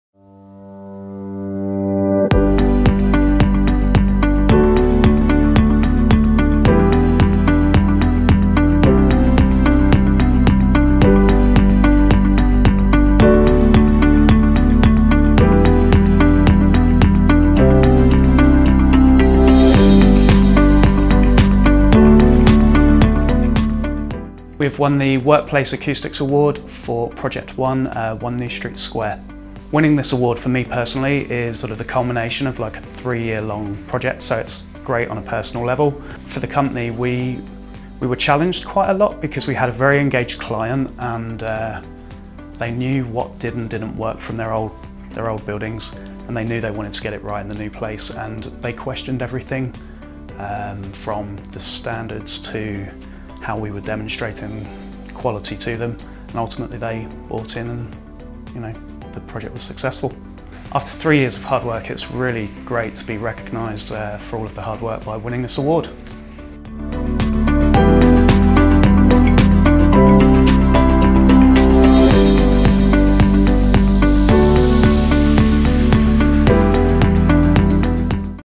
Hoare Lea, Workplace Acoustics - 2019 Acoustic Awards
This award-winning project was celebrated at the Acoustic Awards, organised by ANC, the Association of Noise Consultants, held at the Crowne Plaza, Manchester.

It was presented by renowned television and radio presenter, Maggie Philbin OBE.

For seven years ANC, the voice of professional consultancy in acoustics, noise and vibration, has held the awards to promote and recognise excellence among UK acoustic consultants.

This year the awards showcased the skills of members across six categories. The evening event also saw an award for the best presentation of a shortlisted project.  This had been judged by delegates at the ANC conference, which had taken place earlier in the day.

A distinguished panel of judges comprising academics and professionals, as well as representatives from the sponsor companies, scrutinised the entries.

Dan Saunders, Chair of ANC, said: “The ANC awards highlight the unique skills of our acoustic and noise professionals.

“This year we’ve once again seen entries that represent the dynamic and diverse nature of the whole industry.

“The accolades reflect the ingenuity of our members’ work and highlight the positive impact that good acoustics brings to society overall.”